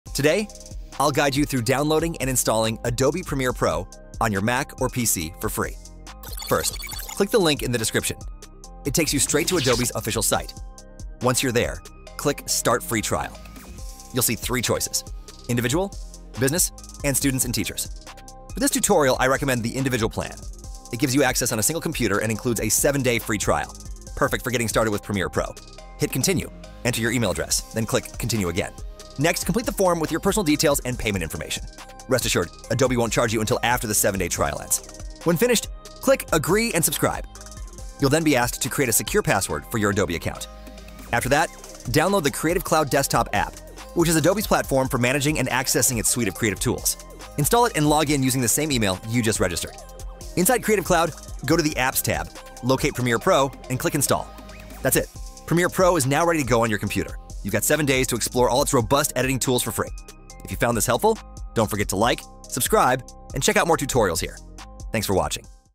How To Download Adobe Premiere PRO For FREE on Pc & Mac (Official Version)
👉 Download  Adobe Premiere PRO For FREE on Pc & Mac

In this video, I’ll show you how to download Adobe Premiere Pro for free on both PC and Mac, guiding you through accessing the free trial and installing it on your system. Today, learn how to download and install Adobe Premiere Pro with this Premiere Pro tutorial for beginners. This video walks you through the process of getting started with video editing using the Adobe Creative Cloud. Follow these steps to begin to edit a video like a pro and find the best video editor for your needs. Adobe Premiere Pro is one of the industry’s leading video editing software programs, offering professional tools for editing, color correction, audio mixing, and much more. Whether you're a beginner looking to edit your first video or a seasoned professional, this free trial gives you full access to all its premium features, so you can explore its capabilities before committing to a subscription.